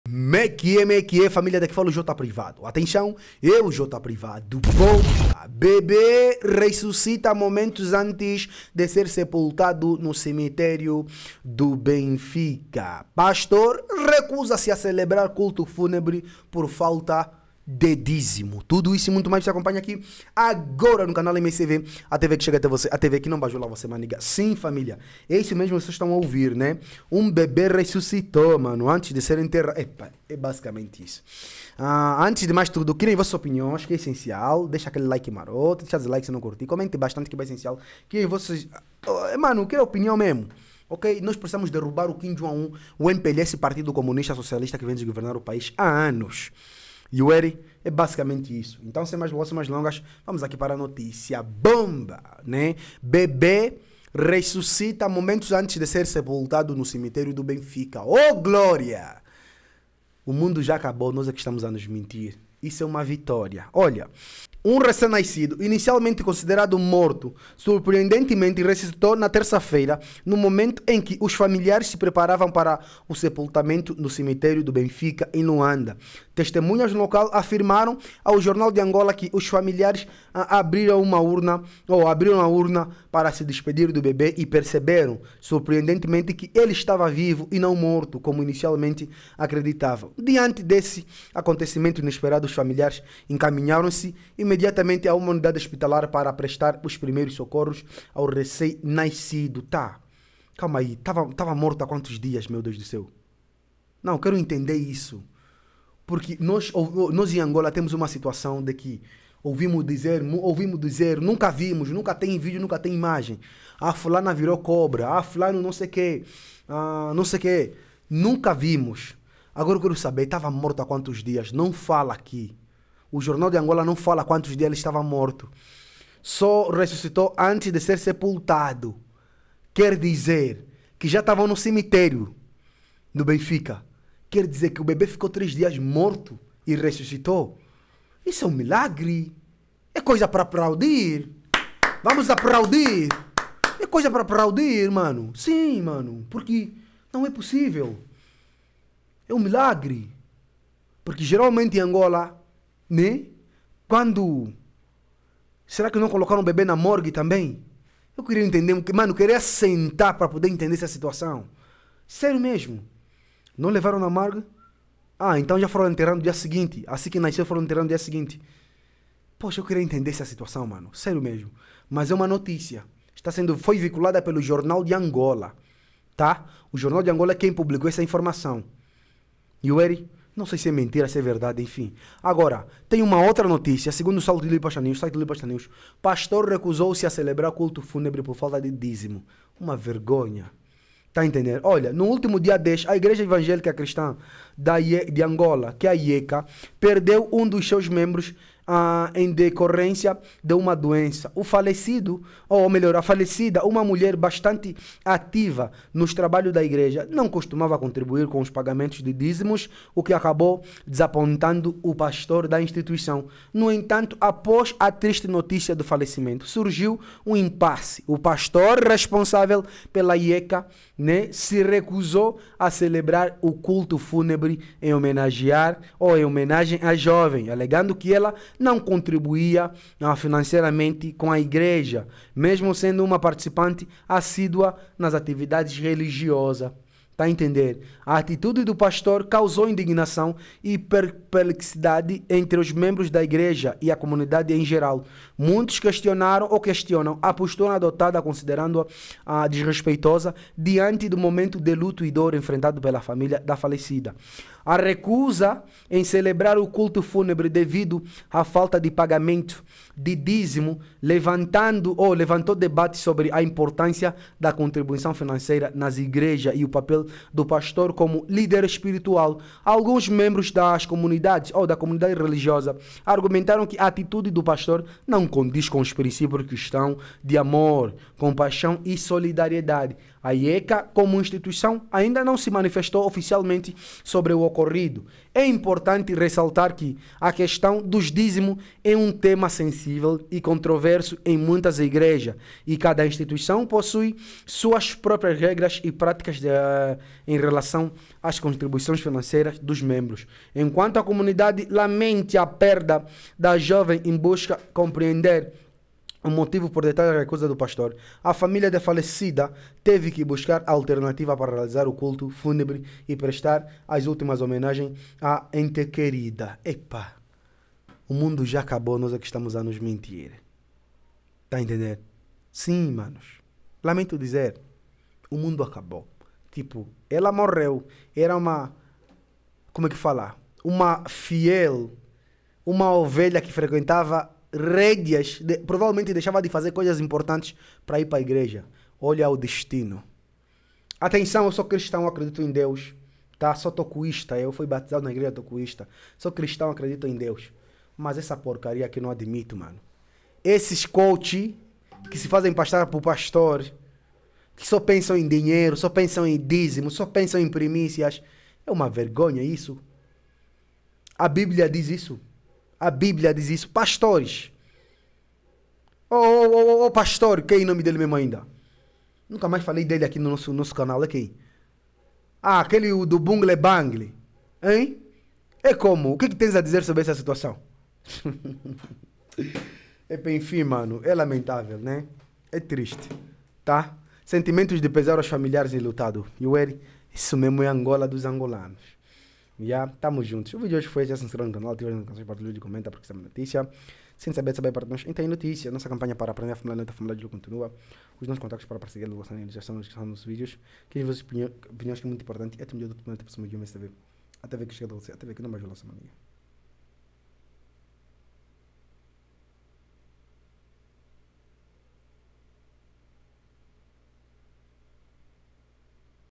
BEBE RESCUCITA MOMENTOS ANTES DE SER ENTERRADO NO CIMITÉRIO DO BENFICA
siga o meu tik Tok